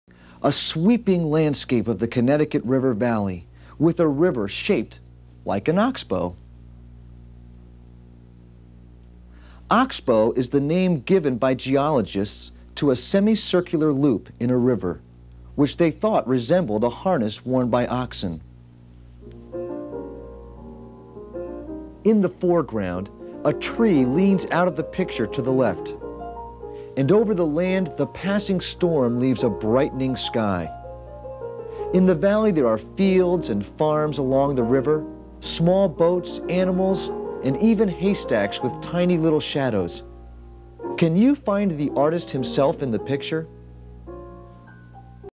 Wilton Art: Introduction to Art and Music in America
Have you ever looked into a painting and heard music? Or maybe listened to some music and visualized a painting? This video correlates the idea of music appreciation and art appreciation by looking at the artist Thomas Cole and the musicc of Edvard MacDowell. Full video is 30 minutes.